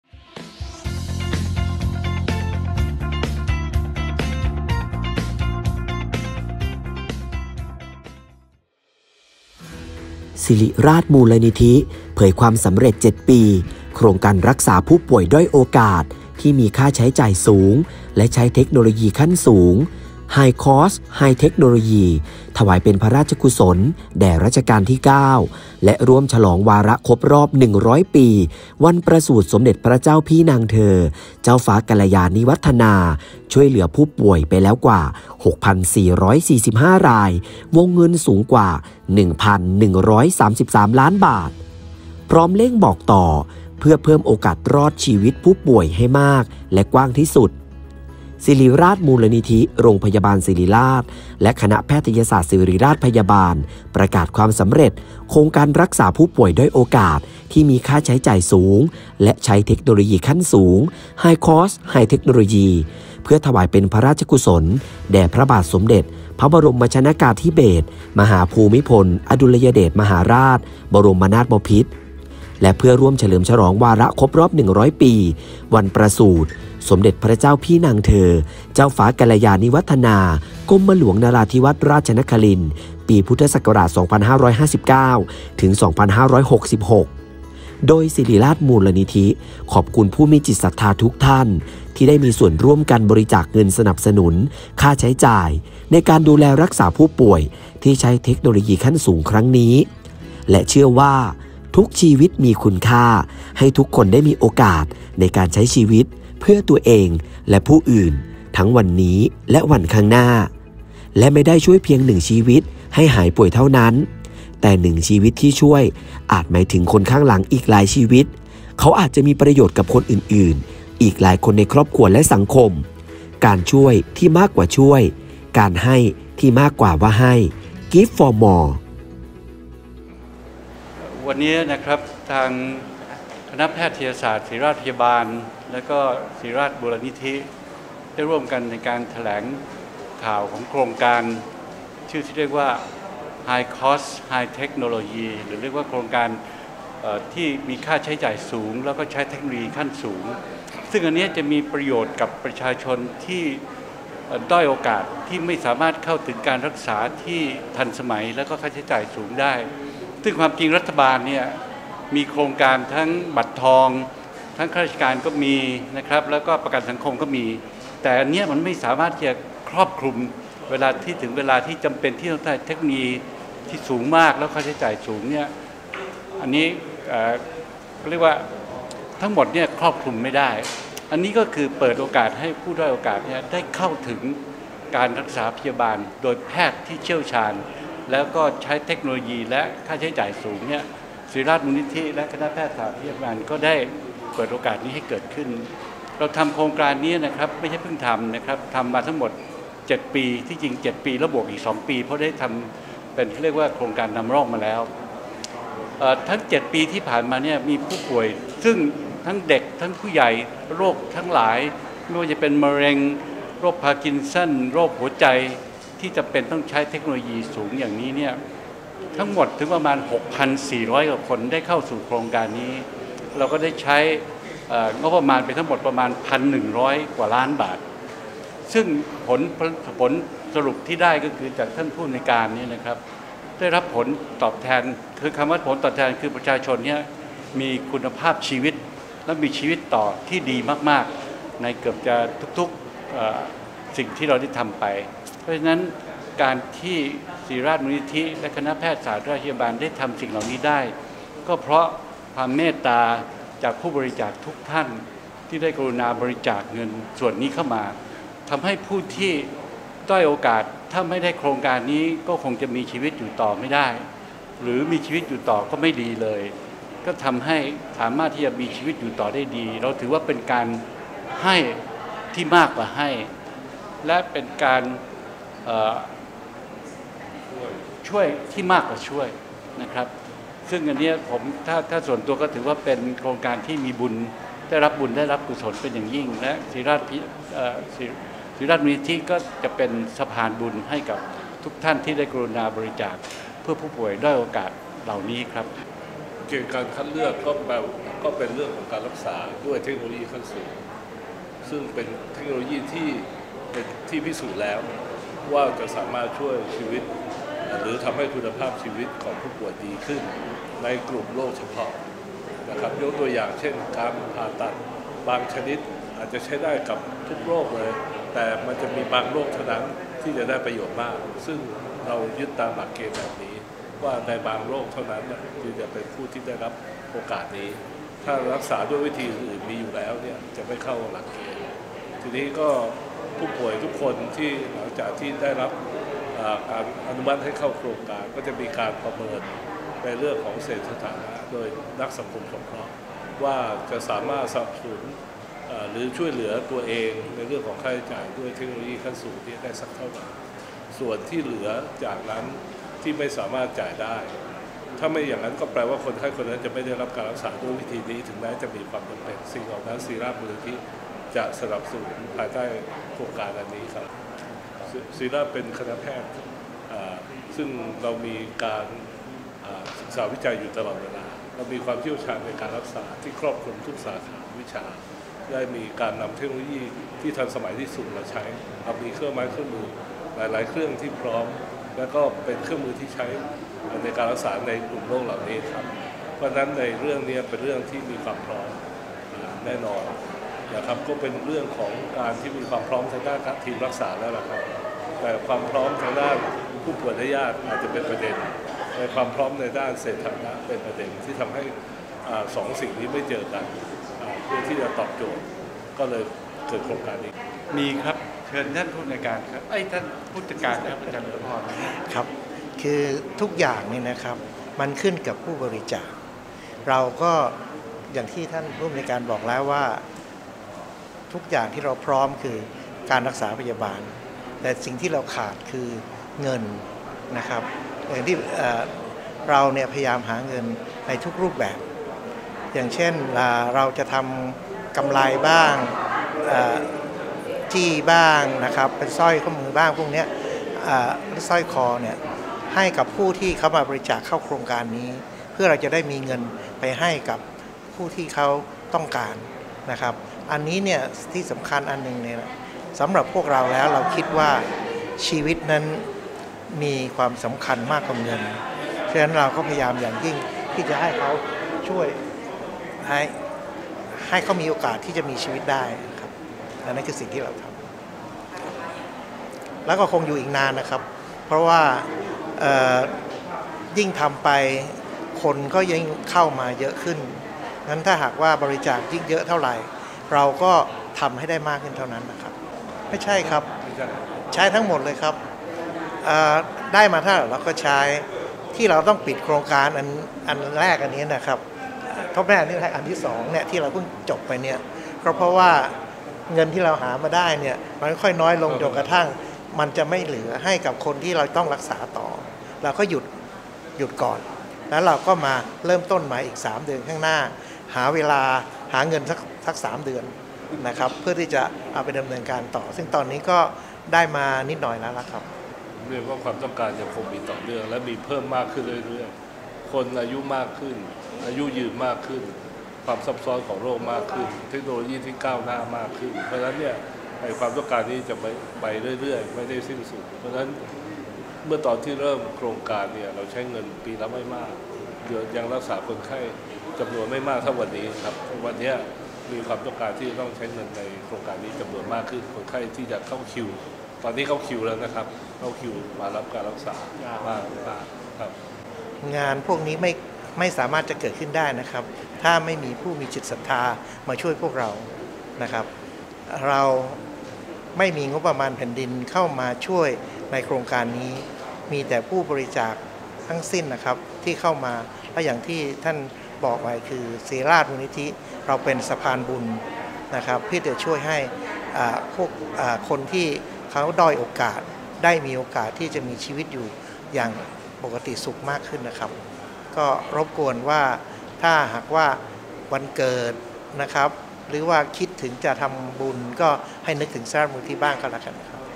ศิริราชมูลนิธิ สำเร็จ 7 ปี “โครงการรักษาผู้ป่วยด้อยโอกาส ที่มีค่าใช้จ่ายสูงและใช้เทคโนโลยีขั้นสูง”
ศิริราชมูลนิธิ โรงพยาบาลศิริราช และคณะแพทยศาสตร์ศิริราชพยาบาล ประกาศความสำเร็จ “โครงการรักษาผู้ป่วยด้อยโอกาสที่มีค่าใช้จ่ายสูงและใช้เทคโนโลยีขั้นสูง” (High Cost High Technology) เพื่อถวายเป็นพระราชกุศลแด่พระบาทสมเด็จพระบรมชนกาธิเบศร มหาภูมิพลอดุลยเดชมหาราช บรมนาถบพิตร และเพื่อร่วมเฉลิมฉลองวาระครบรอบ 100 ปี วันประสูติสมเด็จพระเจ้าพี่นางเธอ เจ้าฟ้ากัลยาณิวัฒนา กรมหลวงนราธิวาสราชนครินทร์ ปี พ.ศ.2559 - 2566 โดย “ศิริราชมูลนิธิ” ขอบคุณผู้มีจิตศรัทธาทุกท่าน ที่ได้มีส่วนร่วมกันบริจาคเงินสนับสนุนค่าใช้จ่ายในการดูแลรักษาผู้ป่วยที่ใช้เทคโนโลยีขั้นสูงครั้งนี้ และเชื่อว่า "ทุกชีวิตมีคุณค่า" ให้ทุกคนได้มีโอกาสในการใช้ชีวิตเพื่อตัวเอง และผู้อื่น ทั้งวันนี้และวันข้างหน้า และไม่ได้ช่วยเพียง 1 ชีวิตให้หายป่วยเท่านั้น แต่ 1 ชีวิตที่ช่วย อาจหมายถึงคนข้างหลังอีกหลายชีวิต เขาอาจจะมีประโยชน์กับคนอื่นๆ อีกหลายคนในครอบครัวและสังคม "การช่วยที่มากกว่าช่วย การให้ที่มากกว่าให้" (Give For More)

ศ.คลินิกเกียรติคุณ นพ.ปิยะสกล สกลสัตยาทร รองประธานกรรมการศิริราชมูลนิธิ คนที่ 1 เปิดเผยว่าทาง ศิริราชมูลนิธิ โรงพยาบาลศิริราช และคณะแพทยศาสตร์ศิริราชพยาบาล ได้ริเริ่ม “โครงการรักษาผู้ป่วยด้อยโอกาสที่มีค่าใช้จ่ายสูงและใช้เทคโนโลยีขั้นสูง” (High Cost High Technology) ตั้งแต่ปี พ.ศ. 2559 เพื่อการรักษาผู้ป่วยด้อยโอกาส ถวายเป็นพระราชกุศลแด่รัชกาลที่ 9 และร่วมเฉลิมฉลองวาระครบรอบ 100 ปี วันประสูติ สมเด็จพระเจ้าพี่นางเธอ เจ้าฟ้ากัลยาณิวัฒนา กรมหลวงนราธิวาสราชนครินทร์ โครงการนี้ได้ช่วยเหลือผู้ป่วยด้อยโอกาสจำนวนมาก ในโรคที่ต้องใช้ค่าใช้จ่ายสูงและจำเป็นต้องทำการรักษาพยาบาลด้วยเทคโนโลยีทางการแพทย์ขั้นสูง ที่ประชาชนทั่วไปเข้าถึงได้ยากและผู้ป่วยเหล่านี้ไม่สามารถรับภาระค่าใช้จ่ายได้ นอกจากนี้ยังมีผู้ป่วยด้อยโอกาสหลายรายเป็นโรคมากกว่า 1 ชนิดที่จำเป็นต้องรับการรักษาและช่วยเหลืออย่างเร่งด่วน อาทิ โรคหลอดเลือดสมองโป่งพอง มะเร็ง เนื้องอกในไต ตับ ปอด และกระดูก โรคปอด โรคหัวใจ โรคระบบทางเดินอาหาร มะเร็งในกระเพาะอาหาร นิ่วในท่อน้ำดี และโรคติดเชื้อร้ายแรงต่าง ๆ เป็นต้น

ซึ่งค่าใช้จ่ายทั้งหมดอยู่ในความรับผิดชอบของศิริราชมูลนิธิและคณะแพทยศาสตร์ศิริราชพยาบาลดังที่ได้กล่าวมาและแรกเริ่มดำเนินโครงการ ฯ ตั้งแต่เดือนมิถุนายน 2559 - มิถุนายน 2561 และหลังจากจบโครงการ ฯ ดังกล่าว มีการขยายระยะเวลาของโครงการ ฯ มาอย่างต่อเนื่อง จนสิ้นสุดในเดือนมิถุนายน 2566 โดยมีวัตถุประสงค์เพื่อสนับสนุนค่าใช้จ่ายในการดูแลรักษาผู้ป่วยที่ใช้เทคโนโลยีขั้นสูงในการรักษา เพื่อถวายเป็นพระราชกุศลแด่รัชกาลที่ 9 และร่วมเฉลิมฉลองวาระครบรอบ 100 ปี วันประสูติ สมเด็จพระเจ้าพี่นางเธอ เจ้าฟ้ากัลยาณิวัฒนา กรมหลวงนราธิวาสราชนครินทร์ รวมระยะเวลา 7 ปี นับตั้งแต่เดือนมิถุนายน ปี พ.ศ. 2559 จนถึงมิถุนายน ปี พ.ศ. 2566
โดยการรักษาผู้ป่วยด้อยโอกาสที่ได้รับการอนุมัติในหลักการจะกระจายไปยัง 13 ภาควิชาและศูนย์ภาคีเครือข่ายย่อยต่าง ๆ อาทิ โครงการการรักษาผู้ป่วยโดยการใส่ลิ้นหัวใจ Pulmonary Valve ผ่านสายสวนหัวใจแทนการผ่าตัด ในผู้ป่วยโรคหัวใจพิการแต่กำเนิด ซึ่งต้องใช้งบประมาณเฉลี่ยสูงถึง 1,050,000 บาท/ราย, โครงการการรักษาโรคหลอดเลือดสมองโป่งพองด้วยการใช้ท่อค้ำยันชนิดพิเศษ (Flow Diverter) ที่ใช้งบประมาณเฉลี่ย 405,000 บาท/ราย, โครงการการช่วยเหลือผู้ป่วยที่มีภาวะการทำงานของปอดและหัวใจผิดปกติรุนแรงเฉียบพลันด้วยเครื่องปอดและหัวใจเทียม ใช้งบเฉลี่ย 300,000 บาท/ราย และโครงการการส่องกล้องทางเดินน้ำดี เพื่อรักษาโรคระบบทางเดินอาหาร เช่น การสลายนิ่วในท่อน้ำดีด้วยเลเซอร์ผ่านกล้อง Digital single operator cholangiopancreatoscopy ใช้งบเฉลี่ย 140,000 บาท/ราย เป็นต้น
โดยเกณฑ์ในการคัดเลือกผู้ป่วยเข้าร่วมในโครงการ ฯ ต้องเป็นผู้ป่วยที่ผ่านการวินิจฉัยโรคตามเกณฑ์ของคณะกรรมการโครงการ ฯ ตัดสินการรักษาพยาบาลที่มีความจำเป็นของผู้ป่วยที่มี High Cost High Technology ที่โรงพยาบาลทั่วไปทำไม่ได้ และเป็นผู้ป่วยที่ได้รับการประเมินทางด้านเศรษฐฐานะแล้วว่าเหมาะ สมจะเข้าร่วมโครงการ ฯ จากนักสังคมสงเคราะห์โรงพยาบาลศิริราช และต้องเป็นผู้ป่วยที่ยินดีที่จะเข้าพักในหอผู้ป่วยสามัญเท่านั้น
ขณะนี้ยังมีผู้ป่วยที่ยังคงมีความจำเป็นต้องใช้ High Cost High Technology อีกจำนวนมาก ศิริราชมูลนิธิ  โรงพยาบาลศิริราช และคณะแพทยศาสตร์ศิริราชพยาบาล จึงได้เปิดโครงการใหม่ภายใต้ชื่อ “โครงการเฉลิมพระเกียรติสมเด็จพระกนิษฐาธิราชเจ้า กรมสมเด็จพระเทพรัตนราชสุดา เจ้าฟ้ามหาจักรีสิรินธร มหาวชิราลงกรณวรราชภักดี สิริกิจการิณีพีรยพัฒน รัฐสีมาคุณากรปิยชาติ สยามบรมราชกุมารี 70 พรรษา เพื่อรักษาผู้ป่วยด้อยโอกาสที่มีค่าใช้จ่ายสูงและใช้เทคโนโลยีขั้นสูง ปี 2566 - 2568” โดยเริ่มโครงการ ฯ ตั้งแต่วันที่ 1 ตุลาคม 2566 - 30 กันยายน 2568

“ขอขอบคุณผู้มีจิตศรัทธา ที่ได้มีส่วนร่วมกันบริจาคเงินสนับสนุนค่าใช้จ่ายในการดูแลรักษาผู้ป่วยที่ใช้เทคโนโลยีขั้นสูงครั้งนี้ และเชื่อว่า "ทุกชีวิตมีคุณค่า" ให้ทุกคนได้มีโอกาสในการใช้ชีวิตเพื่อตัวเอง และผู้อื่น ทั้งวันนี้และวันข้างหน้า และไม่ได้ช่วยเพียง 1 ชีวิตให้หายป่วยเท่านั้น แต่ 1 ชีวิตที่ช่วย อาจหมายถึงคนข้างหลังอีกหลายชีวิต เขาอาจจะมีประโยชน์กับคนอื่น ๆ อีกหลายคนในครอบครัวและสังคม "การช่วยที่มากกว่าช่วย การให้ที่มากกว่าให้" Give For More ให้ทุกชีวิตได้ไปต่อ โดยขอเชิญชวนทำดีร่วมกันด้วยการช่วยกันกระจายข่าวออกไปให้กว้างที่สุด เพื่อเพิ่มโอกาสรอดชีวิตและมีชีวิตของผู้ป่วยให้มากที่สุด” ศ.คลินิกเกียรติคุณ นพ.ปิยะสกล สกลสัตยาทร กล่าวสรุปในตอนท้าย